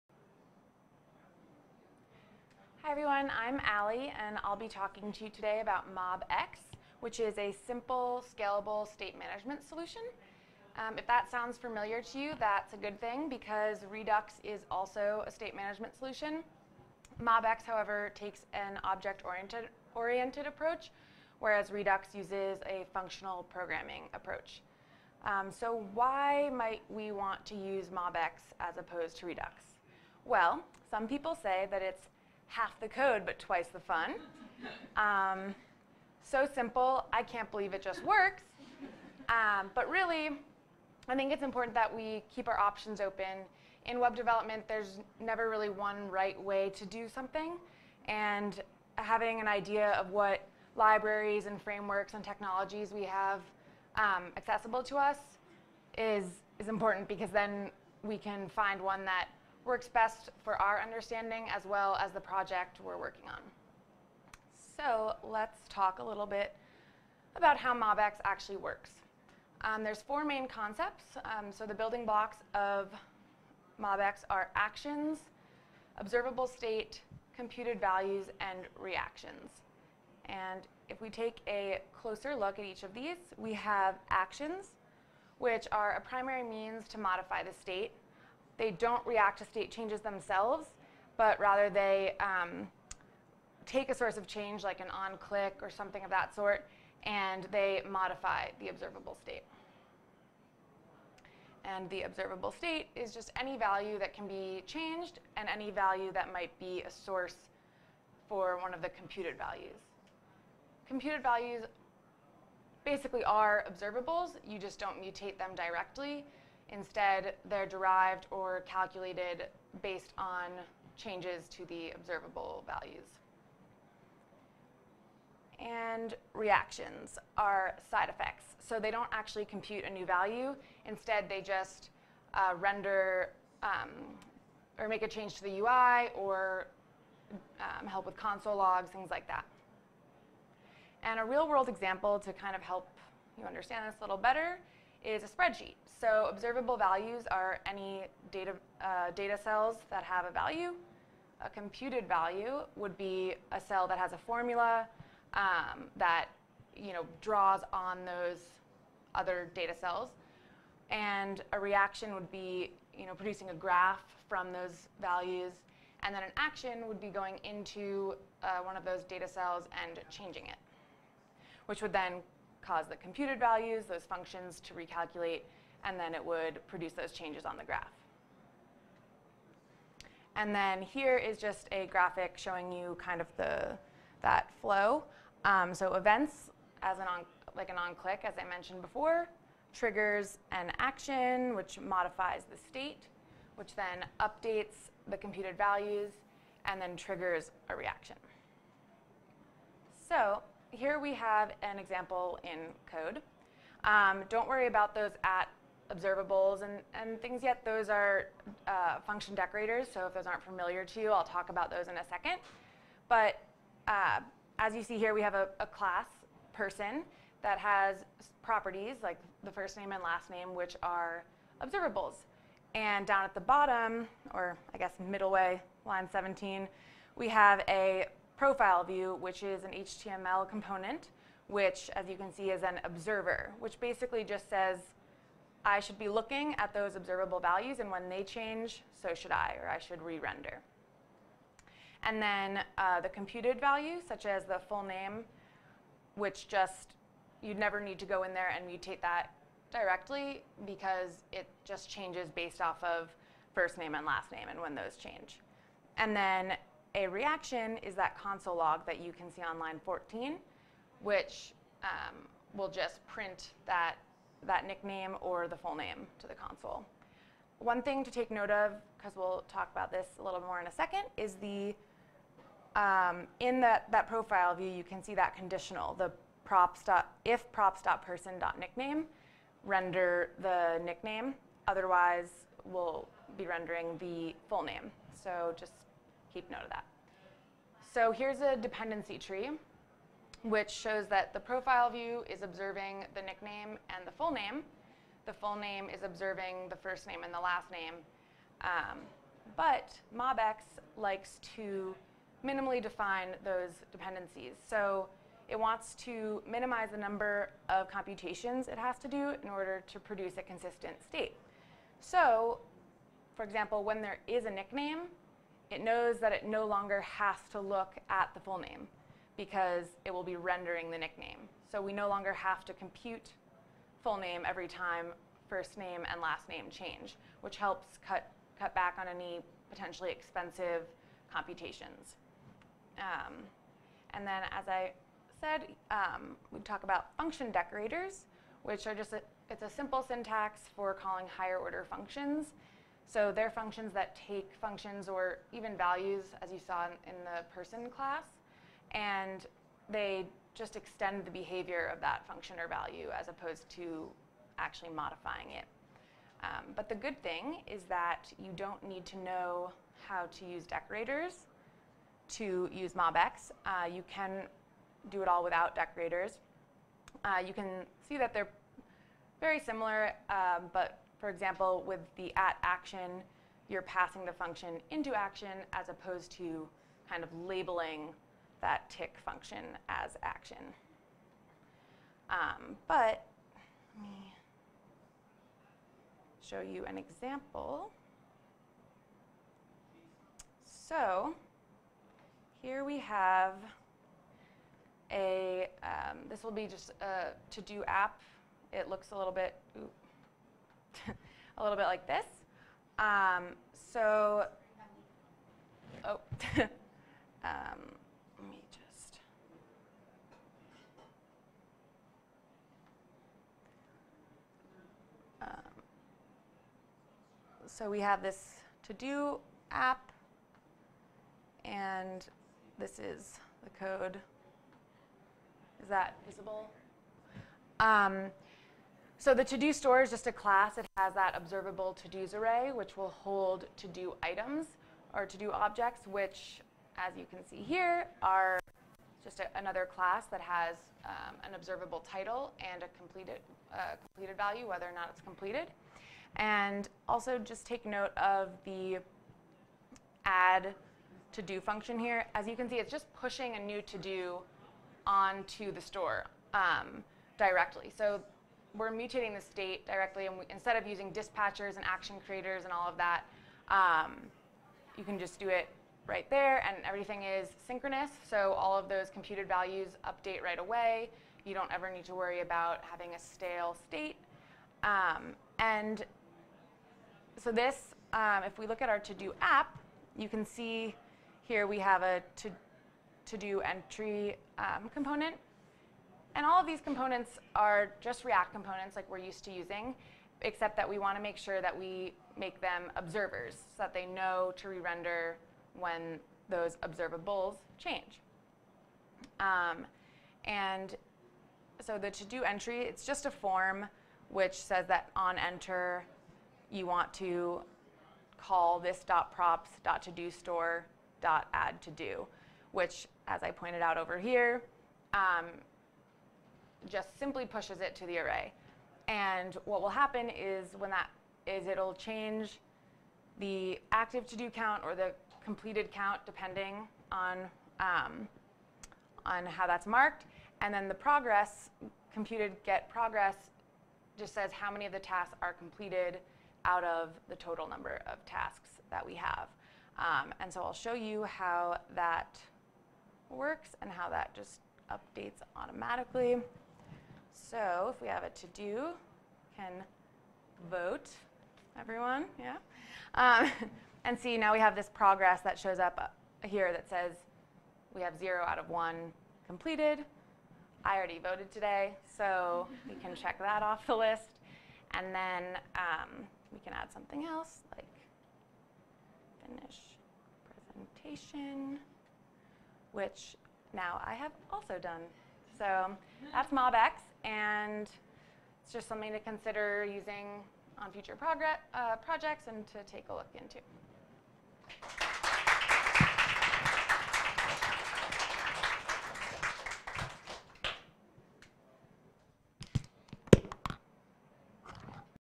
MobX Tutorial - How to Use MobX with React
MobX is a state management library that can be used with React in place of Redux. MobX is open-source and aims to make state management simple and scalable. In this MobX Tutorial, we explain the benefits of an object-oriented approach to state-management and being able to mutate the state directly instead of through asynchronous actions, and show an example that helps illustrate MobX's functionality.

Watch this video to learn:

- What is MobX
- Why use MobX instead of Redux
- The building blocks of MobX and how it can be used with React